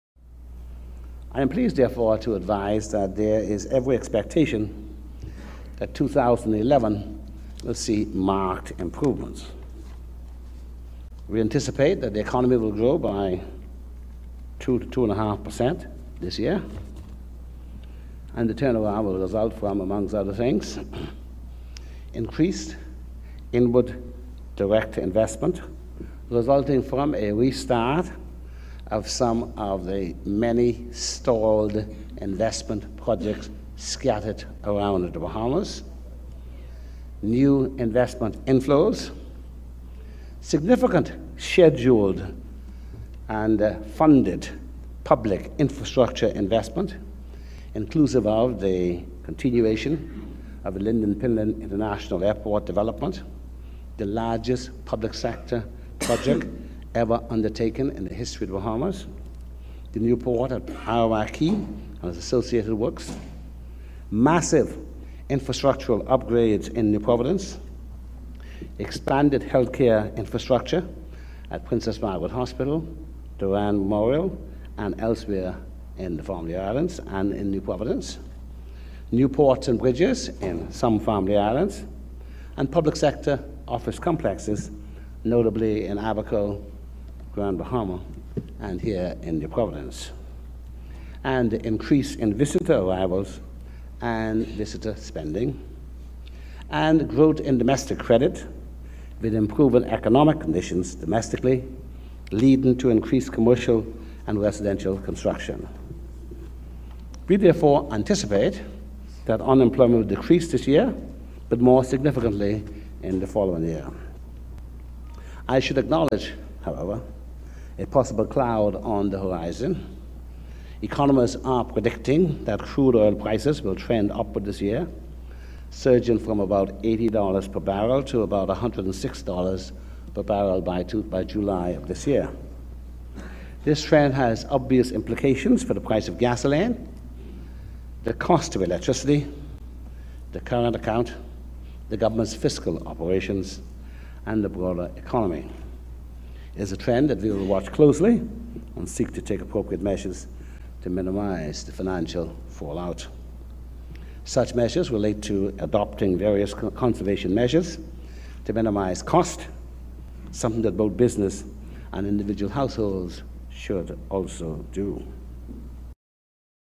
PM: Economy Expected to Grow by 2.0 to 2.5% This Year
Prime Minister the Rt. Hon. Hubert Ingraham announces domestic prospects for the year during his address to the 2011 Bahamas Business Outlook in Nassau.

FULL TEXT OF PM INGRAHAM'S REMARKS CAN BE FOUND ON THE FNM'S OFFICIAL